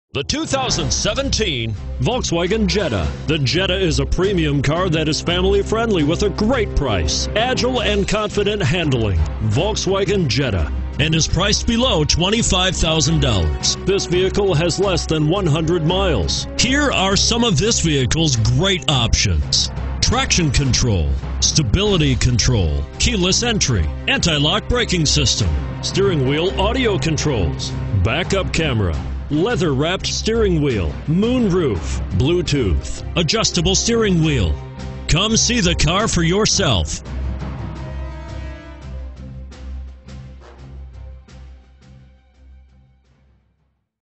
2017 Volkswagen Jetta Colmar PA 17V1098
2017 Volkswagen Jetta 1.4T SE

You'll love this 2017 Volkswagen Jetta. This is a 4dr car you'll want to take home. With 10 miles, it features Automatic transmission and an exterior color of Pure White. Call and get in touch with North Penn Volkswagen directly, and be the first to open the 4dr car door today!